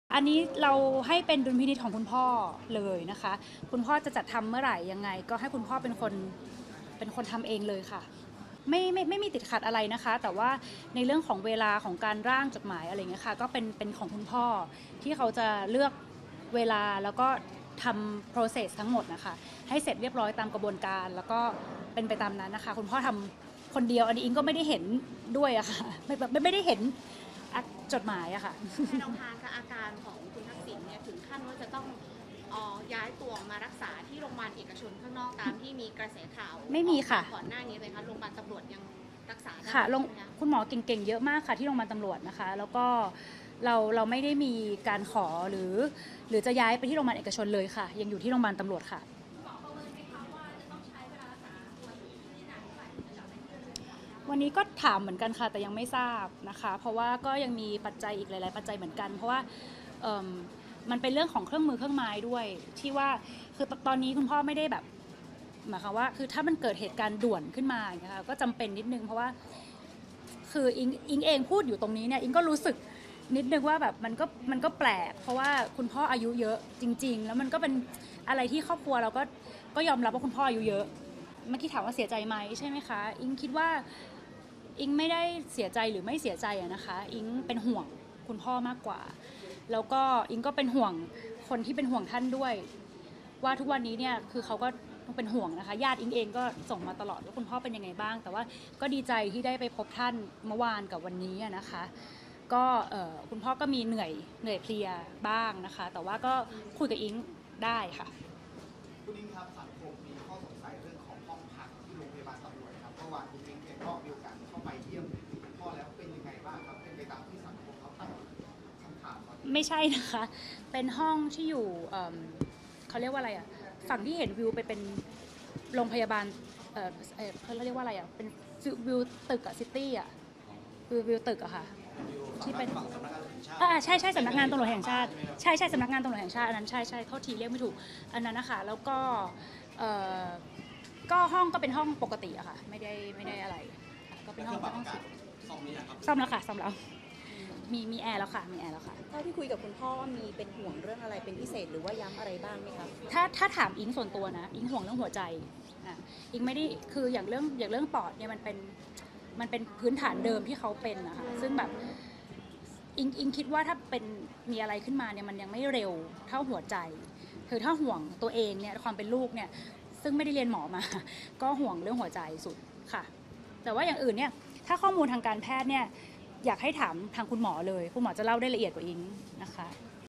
"อุ๊งอิ๊ง" ปัดย้ายพ่อไปเอกชน เผยห่วงเรื่องหัวใจ โยน "ทักษิณ" ขอพระราชทานอภัยโทษเอง
"แพทองธาร" เผยอาการ “พ่อทักษิณ” เครียด-อ่อนเพลีย เชื่อยังปรับตัวไม่ได้ ที่บอกน่าห่วงเพราะเคยติดโควิดเข้าไอซียู 9 วัน ยันไม่เคยขอย้าย รพ.เอกชน เผยแอร์ห้องพักซ่อมแล้ว ให้เจ้าตัวตัดสินใจเองเรื่องขอพระราชทานอภัยโทษ